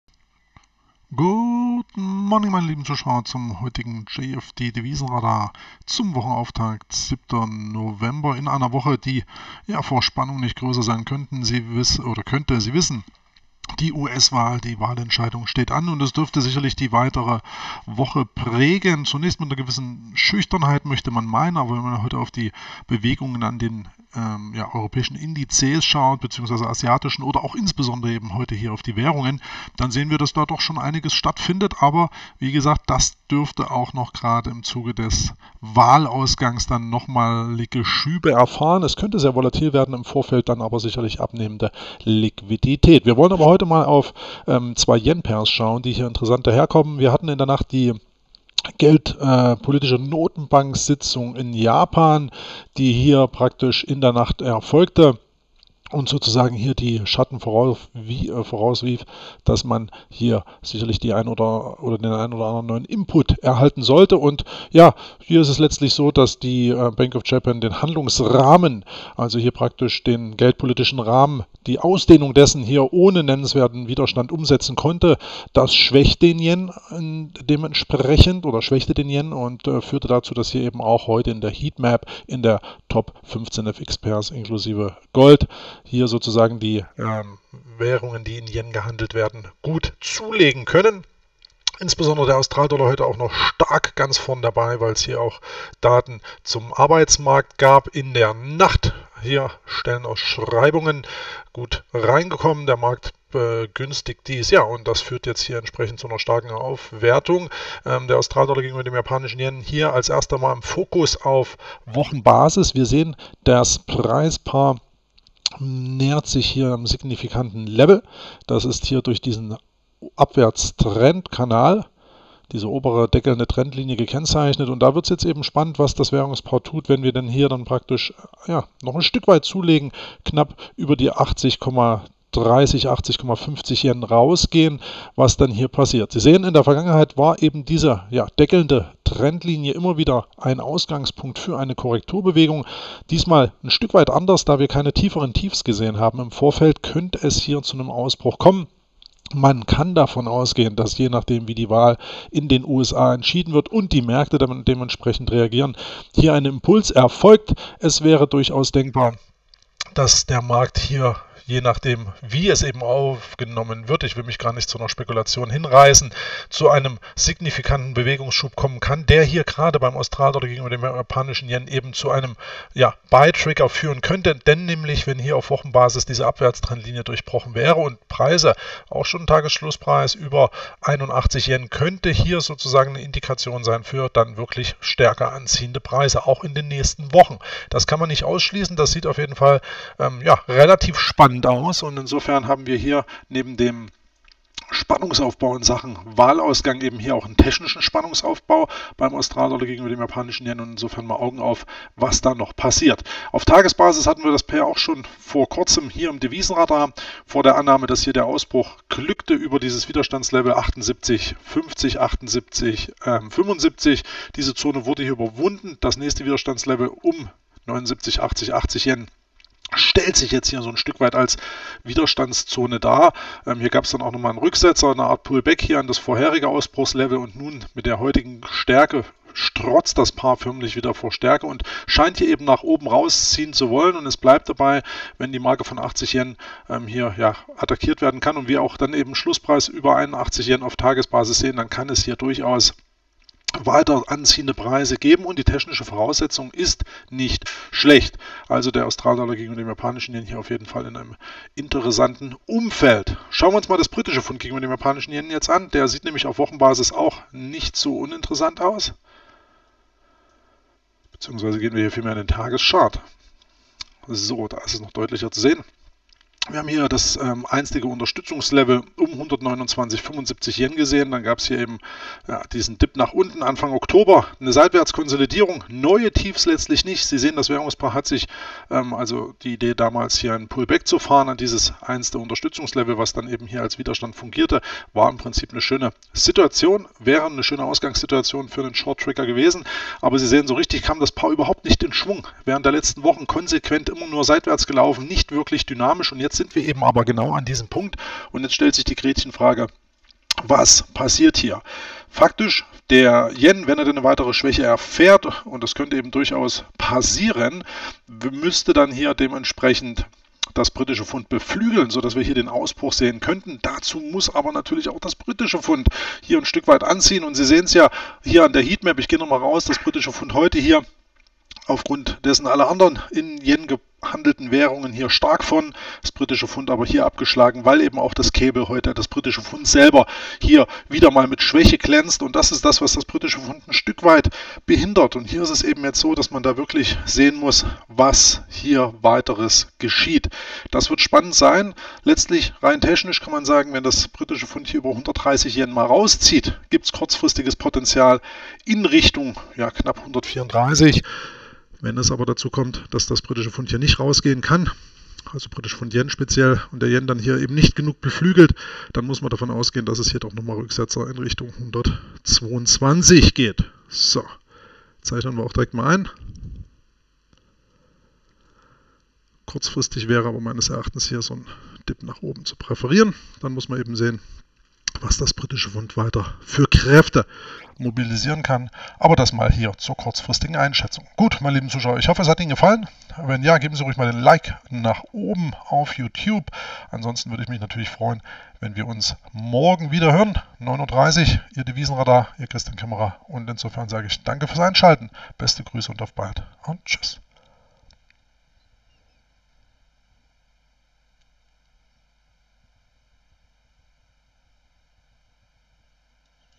JFD Devisenradar: AUD/JPY und GBP/JPY zum Wochenauftakt
Aufgrund des heutigen Protokolls der geldpolitischen Notenbanksitzung in Japan erfährt der YEN sichtliche Schwäche. Die heute im Fokus stehenden Pairs notieren dabei an interessanten Marken.